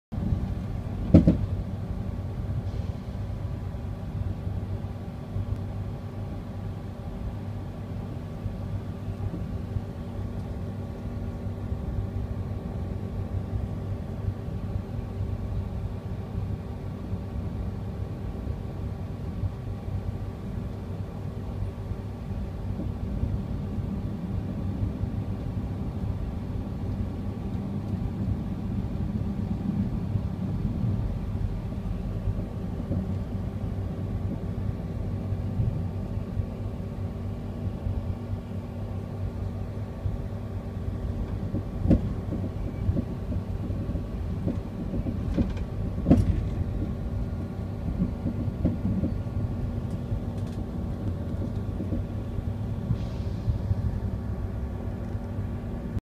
Gotthard from the train
On the train from Zurich to Milan, through Gotthard Switzerland